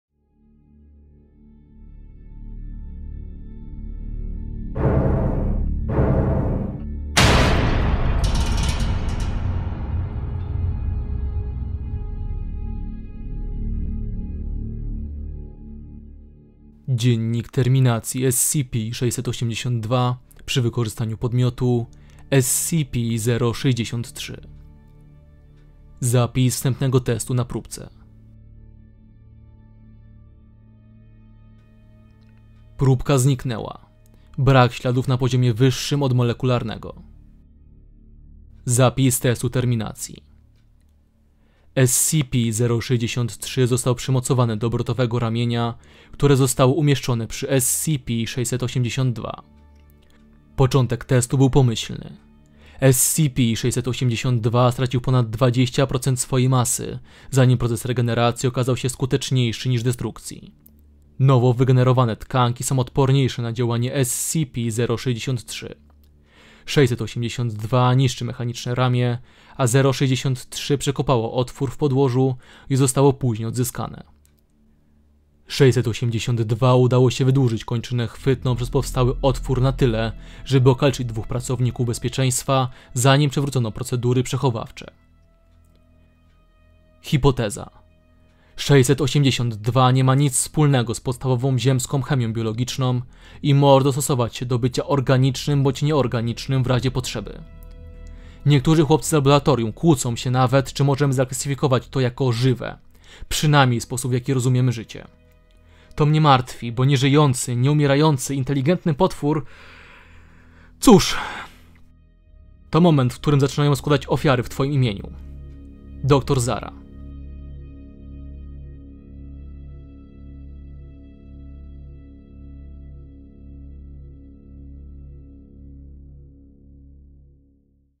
SCP-682 vs SCP-063 - Dziennik terminacji
Czy "Najlepsza szczoteczka do zembów" może pokonać SUPER NAJLEPSZĄ JASZCZÓRKĘ NA ŚWIECIE? Pora się przekonać.